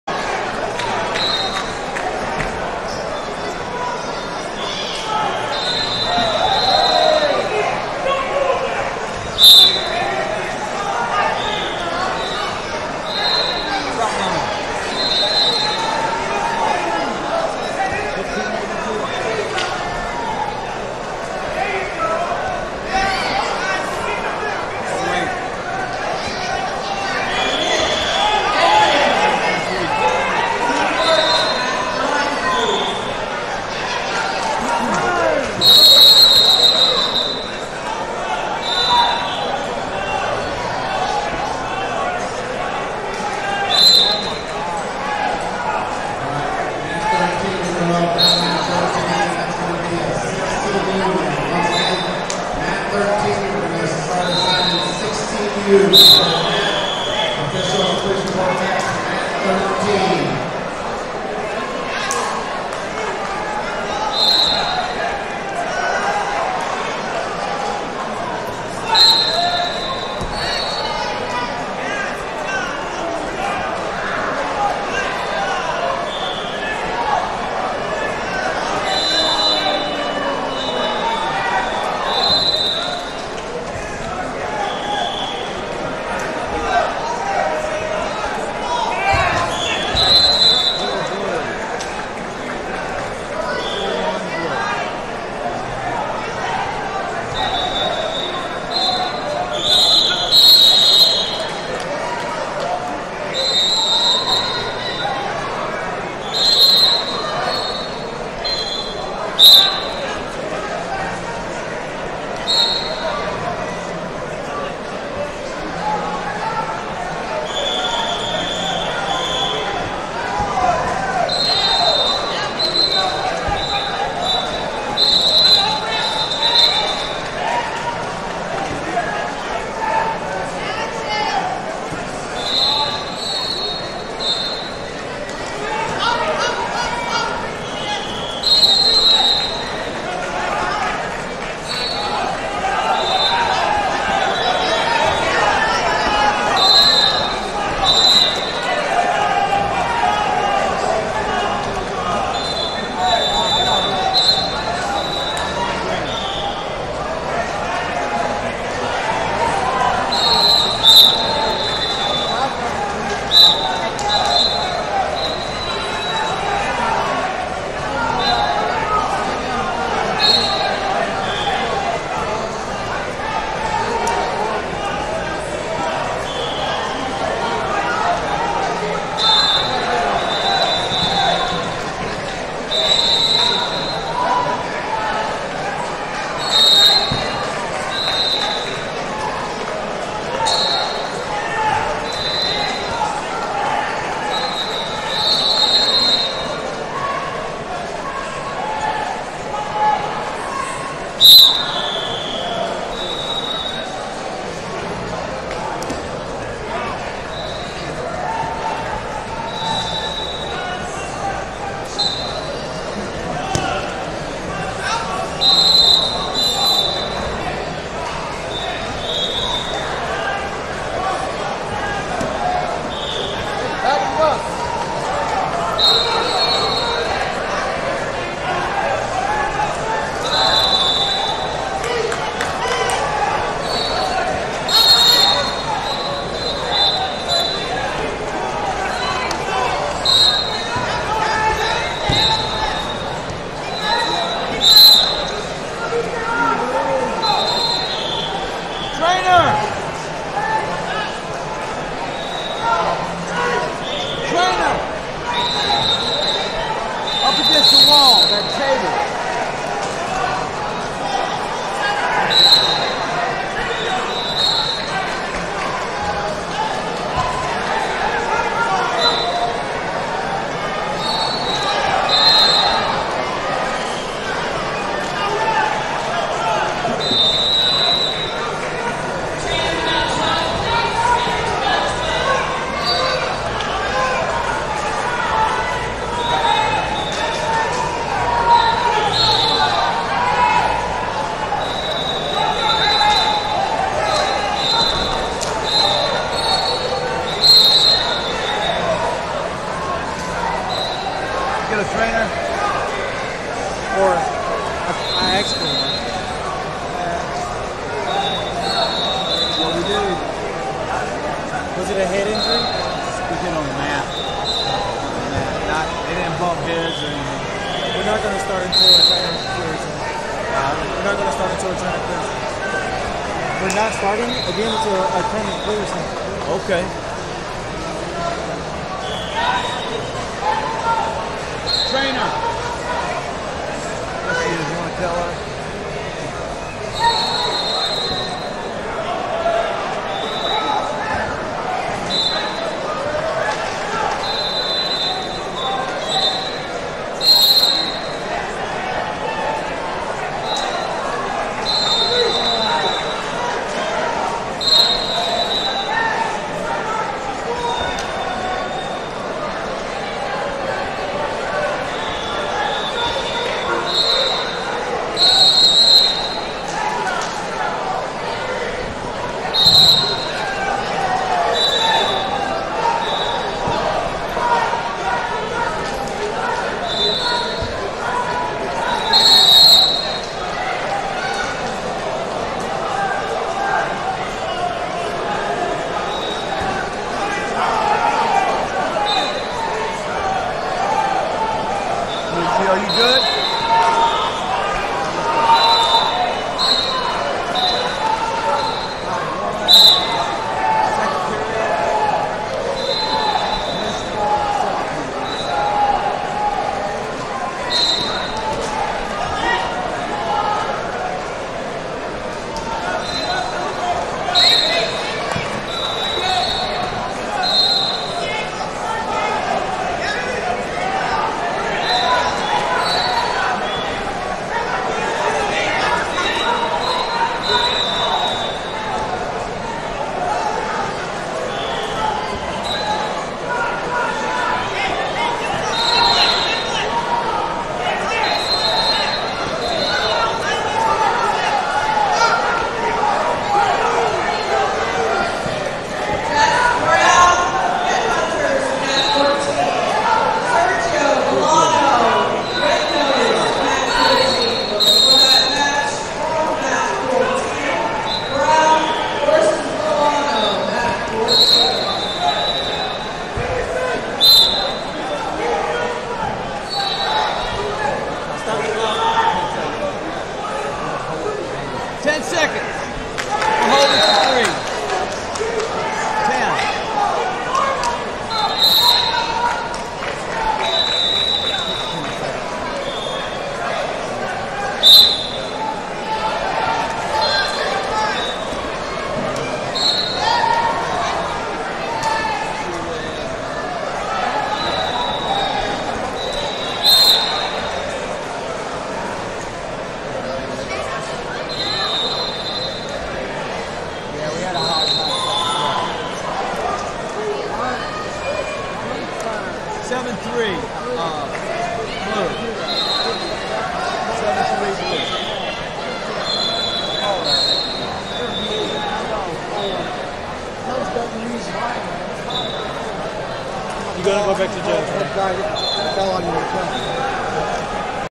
145 Lbs Consi Of 8 #1 - Casper Stewart, Curby 3 Style Wrestling Club Vs Griffin Walizer, M2 Traini
2022 Phil Portuese Northeast Regional Championships - 145 Lbs Consi Of 8 #1 - Casper Stewart, Curby 3 Style Wrestling Club Vs Griffin Walizer, M2 Training Center Fb29